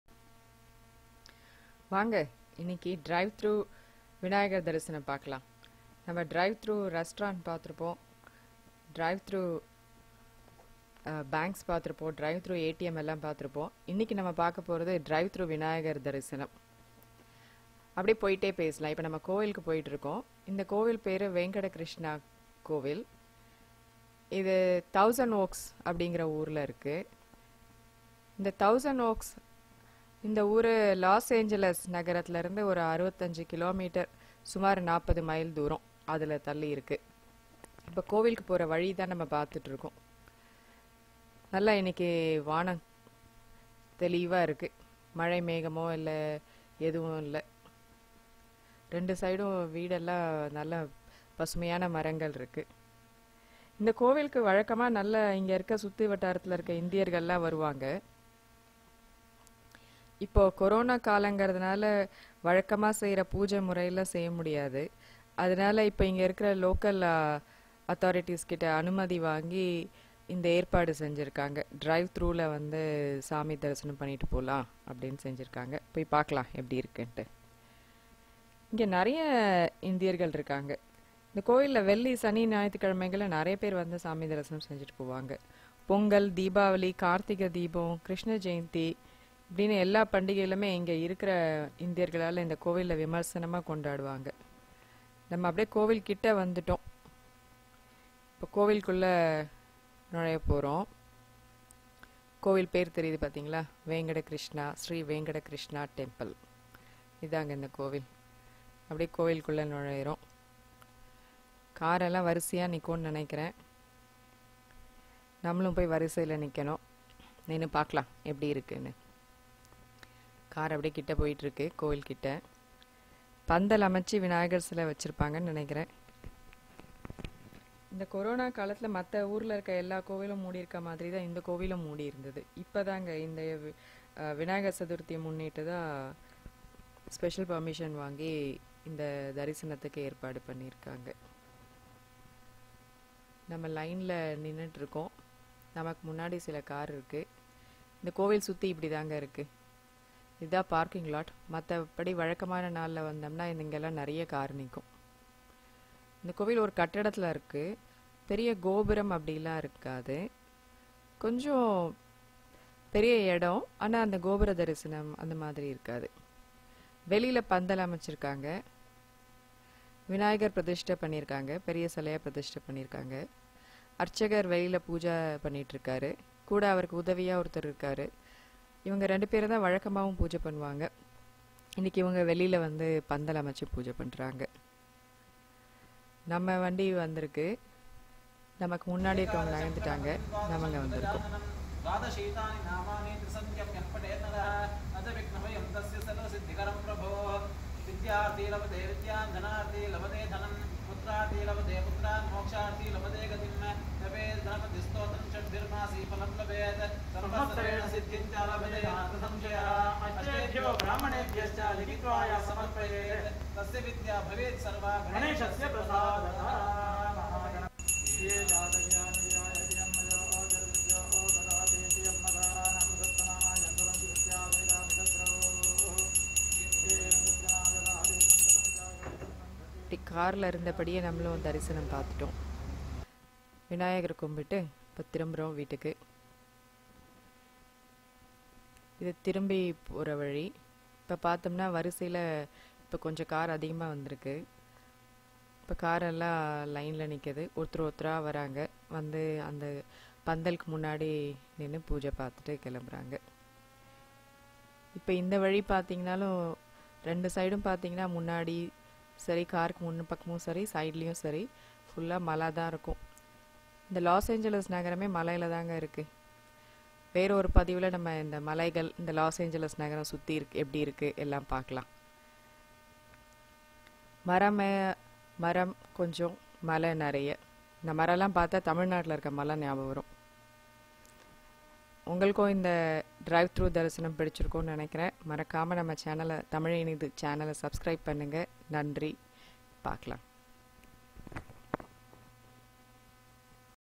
அமெரிக்காவில் டிரைவ் thru விநாயகர் தரிசனம் / Drivethru Vinayagar Darshan in America / Tamizh Inidhu
அமெரிக்காவில் டிரைவ் thru விநாயகர் தரிசனம் / Drive thru Vinayagar Darshan in America /  USA Tamizh Inidhu

Joins us for a drive thru Vinayagar Darshan organized by a temple near Los Angeles , California, USA. This was conducted for Vinayagar Chathurthi celebrations in 2020.  As devotees could not go to the temple during this pandemic, a drive-thru darshan was organized. We will see the beautiful location of the temple and enjoy the darshan.